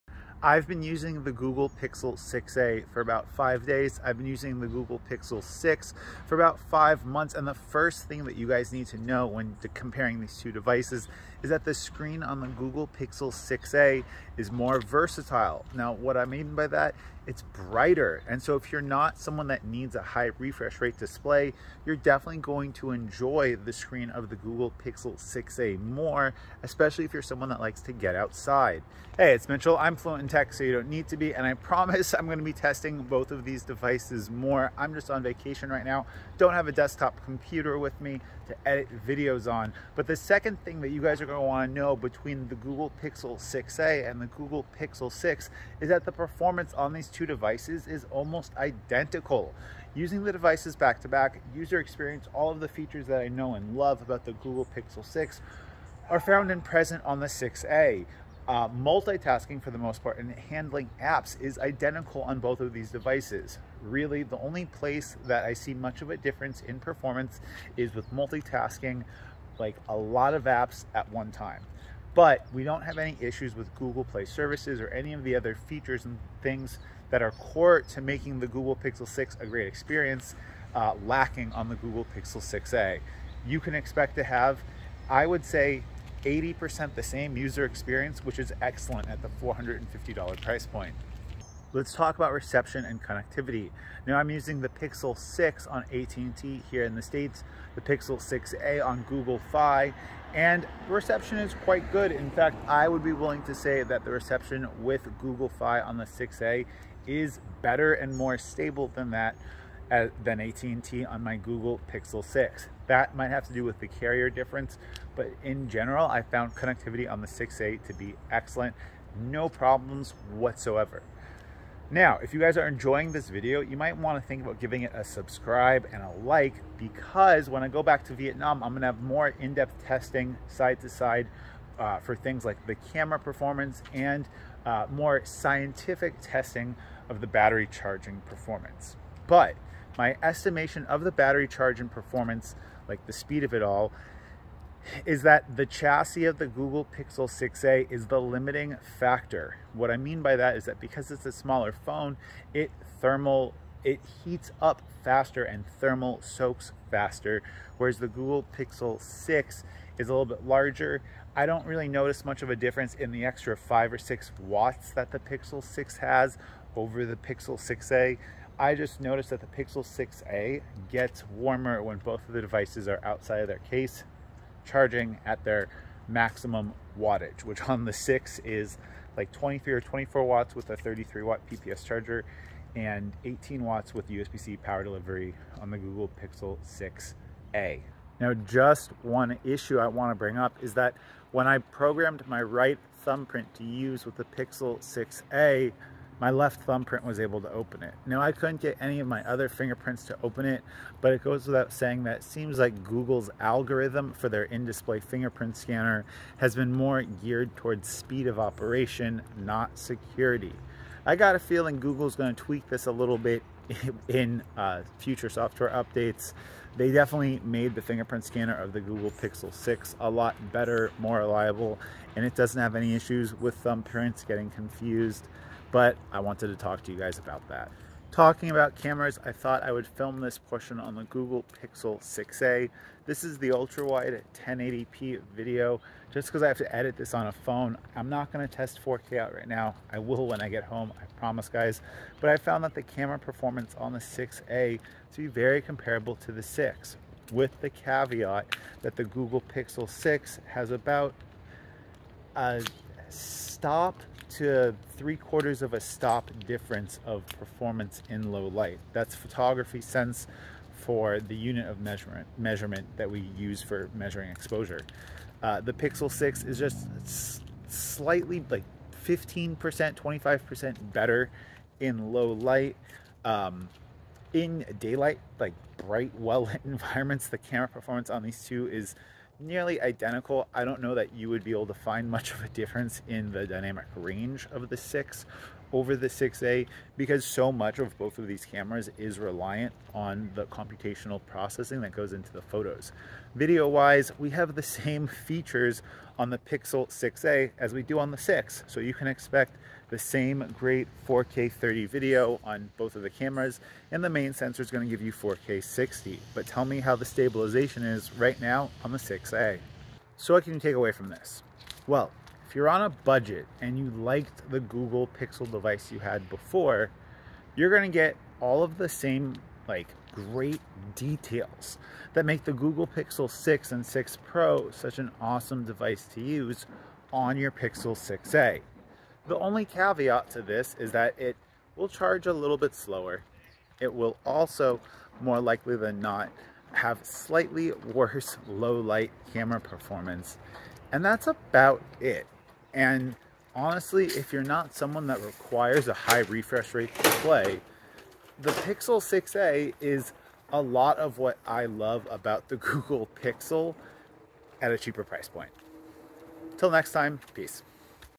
Google Pixel 6A VS Google Pixel 6, It's Not What You Think
If we compare the Google Pixel 6A versus the Google Pixel 6 . but a lot of people looking at the Google Pixel 6 would probably be just as happy with the Google Pixel 6A. in fact, I think some people in some circumstances might find the Google Pixel 6A a better phone than the Google Pixel. 6. The Google Pixel 6A has a brighter screen than the Google Pixel 6. even though the pixel 6A does not have a higher refresh rate. some people like myself would prefer a phone like the Google Pixel 6A because it has a brighter screen and we personally don't care about the refresh rate. another group that might like the Google pixel 6A instead of the Google pixel 6 would be people that don't need to have the best camera and low light. The camera of the Google pixel 6A performs almost identical to that of the camera of the Google Pixel. 6. during normal daylight conditions and even Twilight hours. You really don't see the difference between the Google Pixel 6 and the Google Pixel 6A camera until you get into lower light conditions, which in my opinion makes the Google Pixel 6A a phenomenal value for most consumers that just want to have a device that works extremely well with all of Google services gets good battery life and has a very capable camera